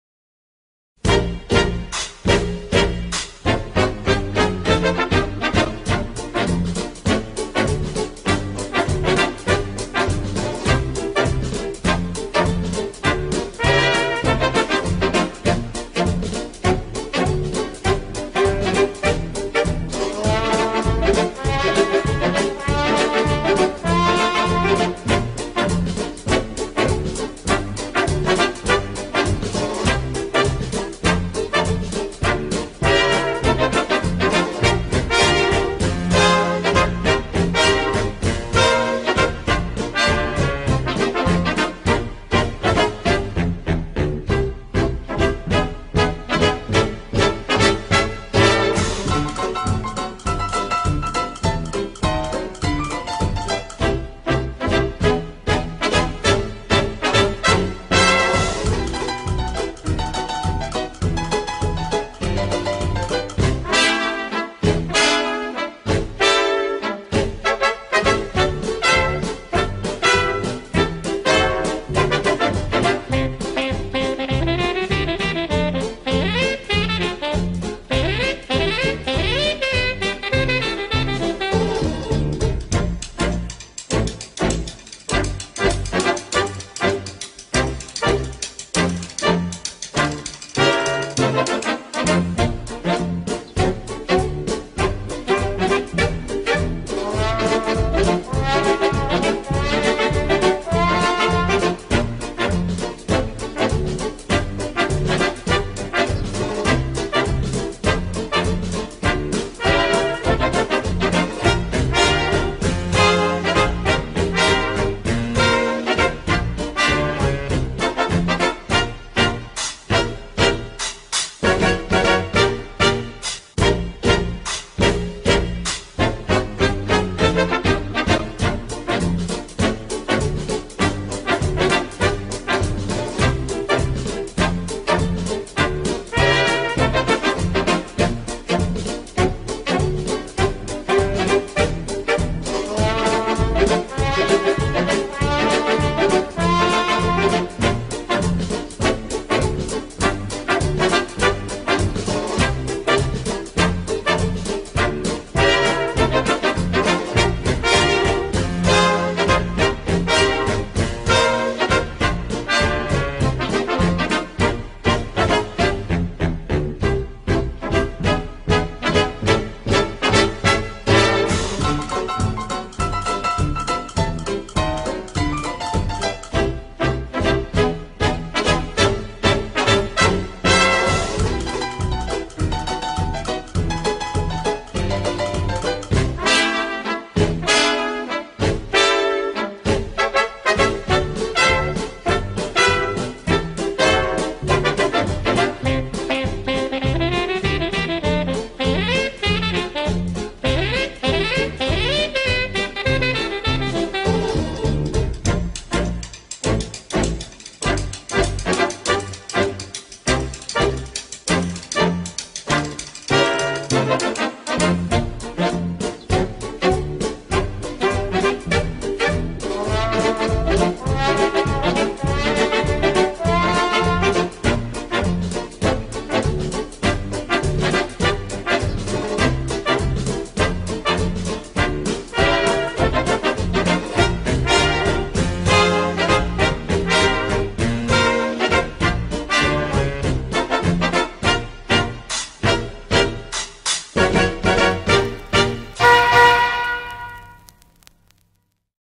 Veteráni Chlumčany 2014
5. přehlídka elegance automobilů Tatra v Chlumčanech, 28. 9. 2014.